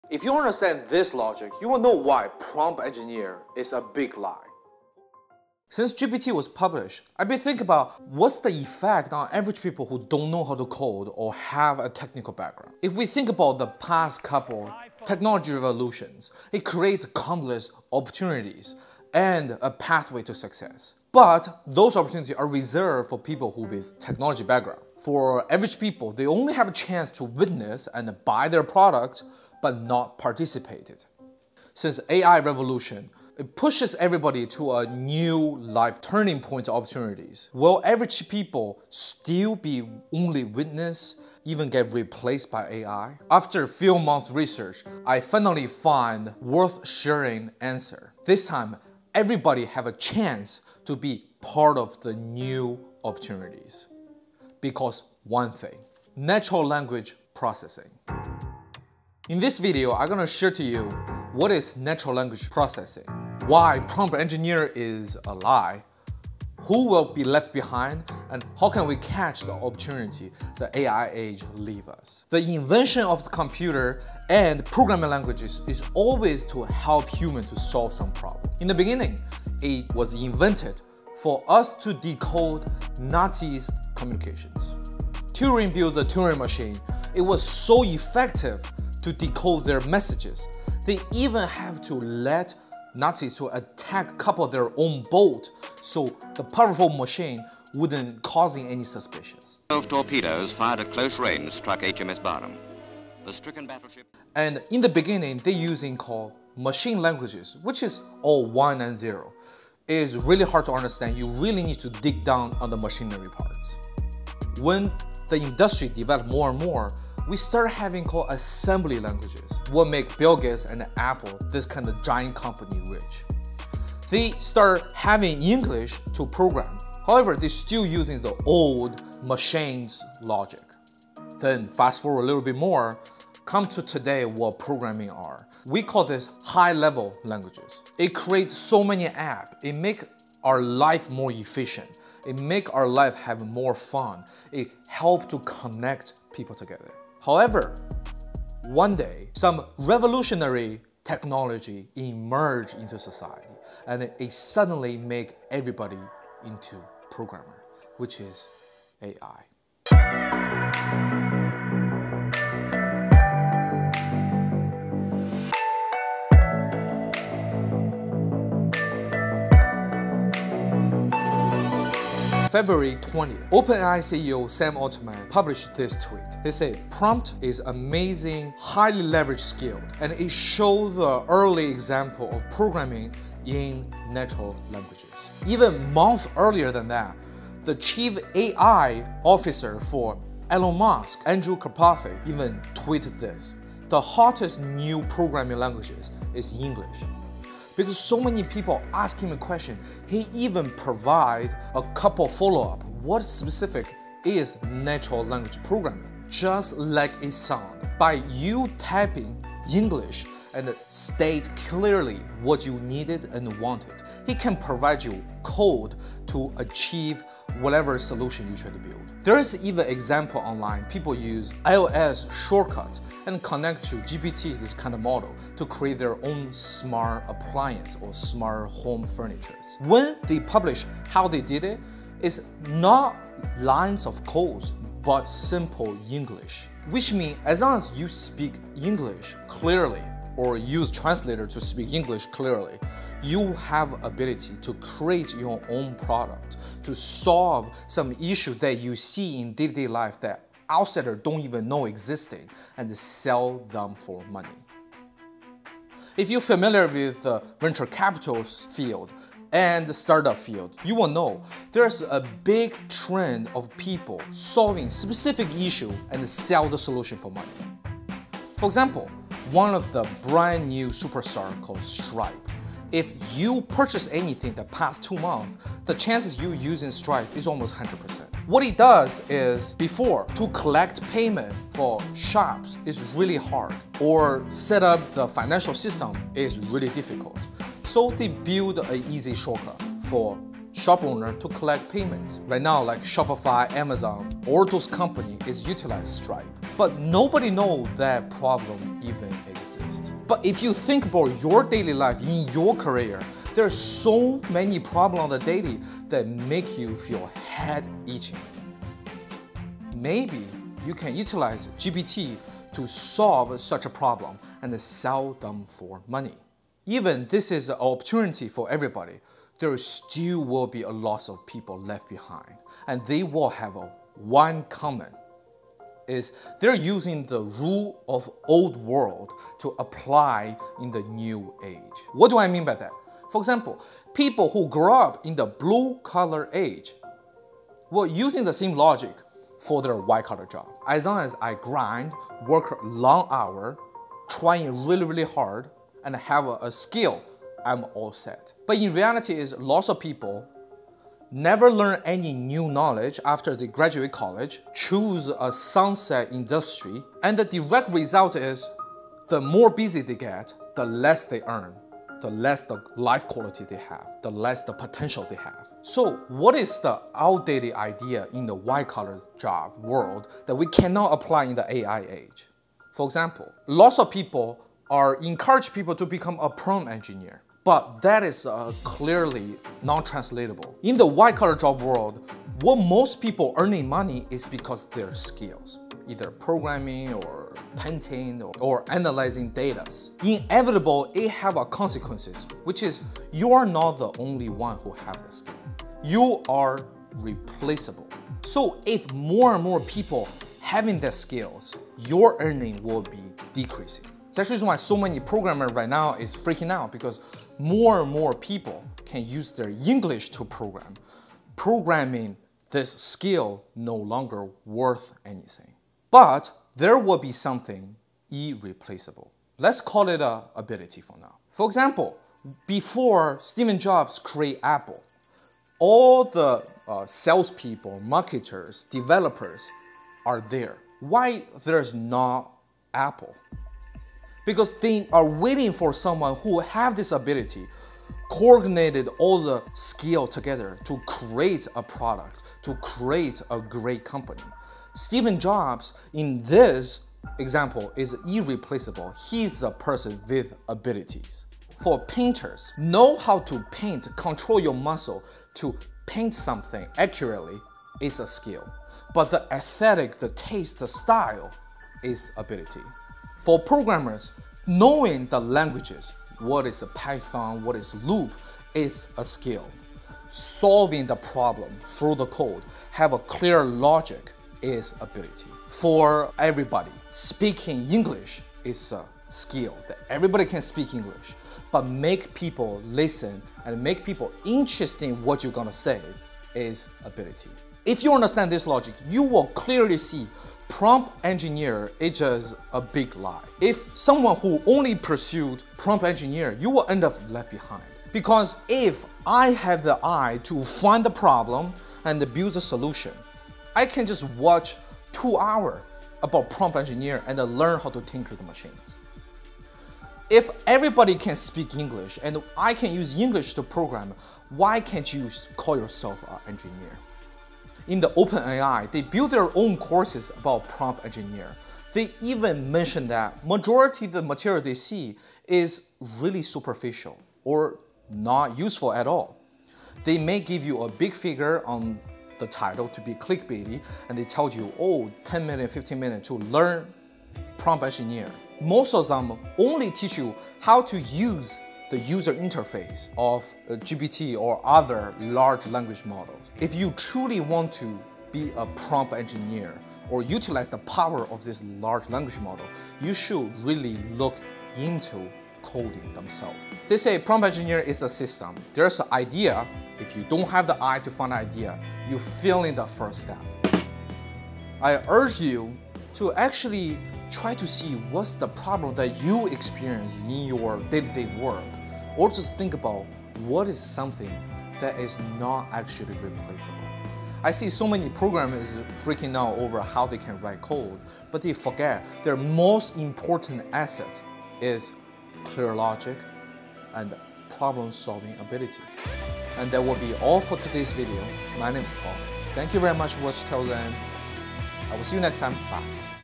This Is Why Prompt Engineering Is A Lie
Today we discuss AI once again, this time from the perspective of people who do not have technical background. Industrial and technical revolutions so far were unkind to people outside of the industry, but I will discuss why this time AI will be the great equalizer and how all of us can access great opportunities for our futures.